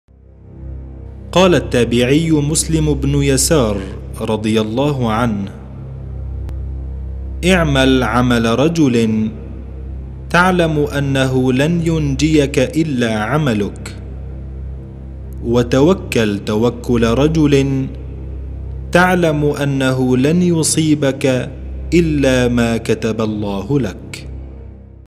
نفحات العلماء -21-
نفحات العلماء:
قال التابعي مُسْلمُ بن يسار رضي الله عنه: اعمَل عَمَل رجلٍ تَعلم أنه لن ينجيَك إلا عملُك، وتوكَّلْ توكُّلَ رجل تعلمُ أنه لن يصيبَك إلا ما كَتب الله لك.
وعلى التردد: 11595 عمودي، الترميز 27500 ، معامل التصحيح 3/4